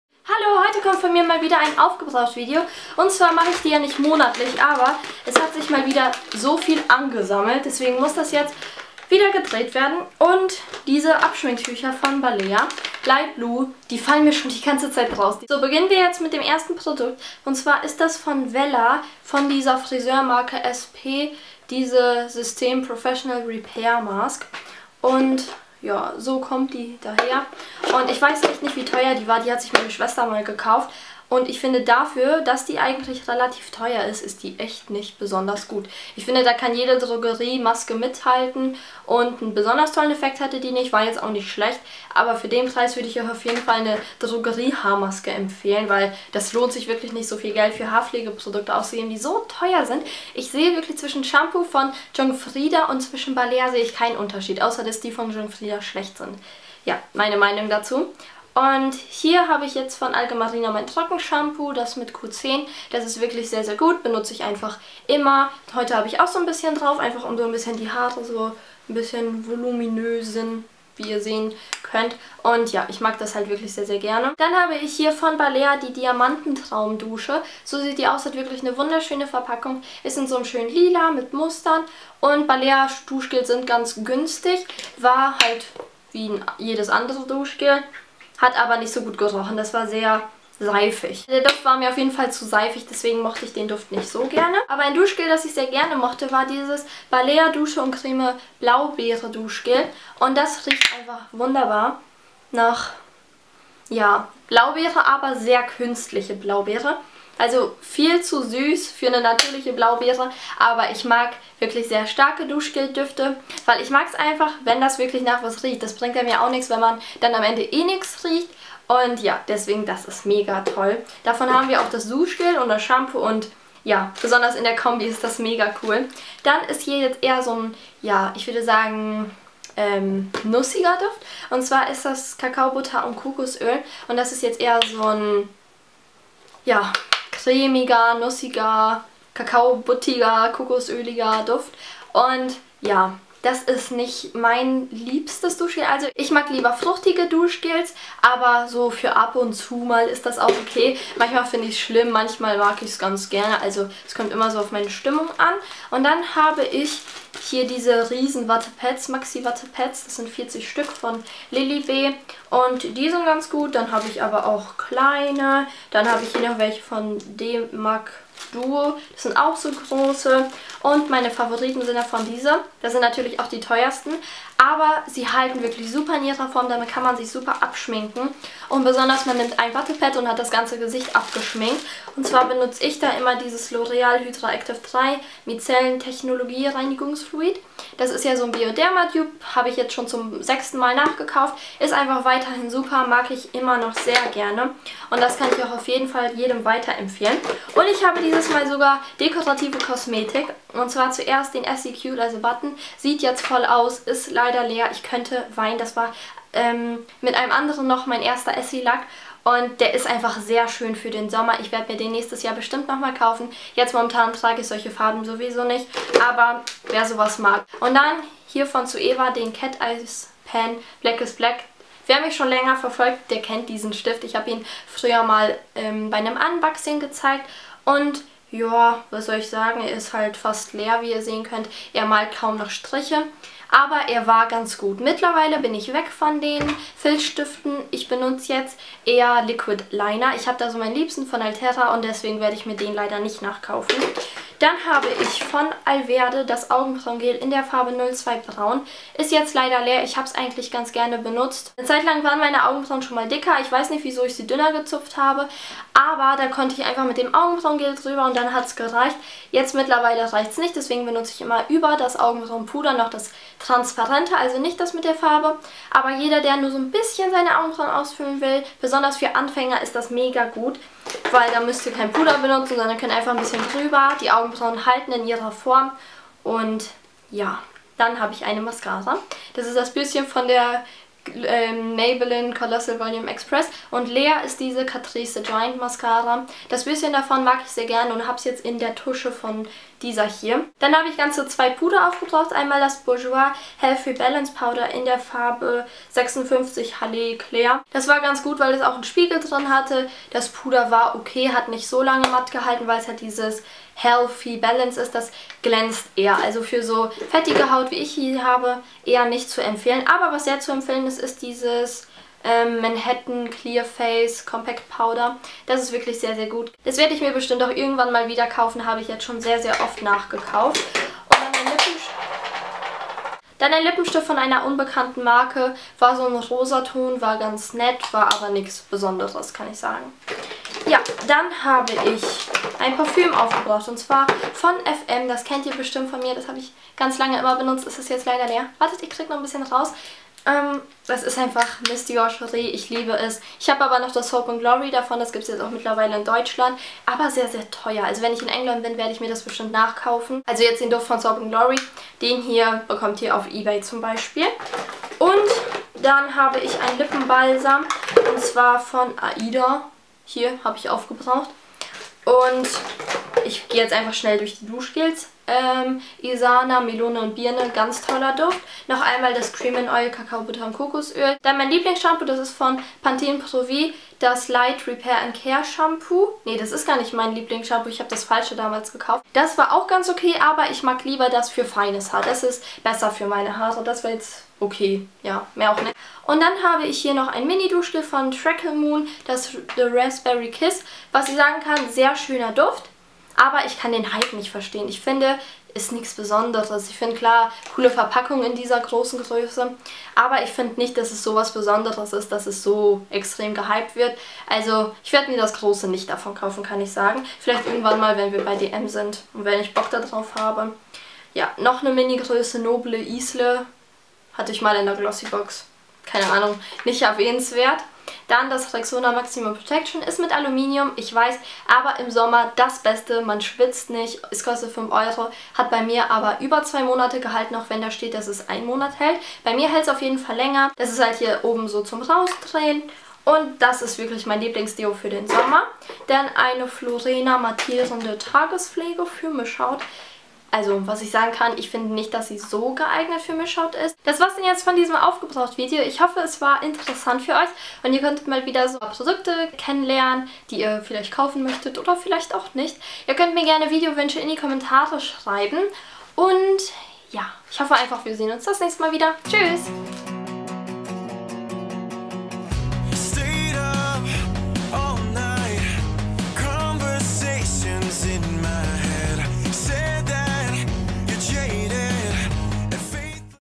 Aufgebraucht! Meine Empties #13
VIDEOFRAGE: Möchtet ihr von Mittwoch bis Sonntag tägliche Videos? :)

In diesem Video zeige ich euch mal wieder, was ich aufgebraucht habe.

LUST AUF MEHR VIDEOS?

ICH TRAGE:
Jacke: New Yorker

Ihr wollt immer auf dem neusten Stand sein? Folgt mir!
★ Gefilmt mit der Canon eos 600d und bearbeitet mit iMovie.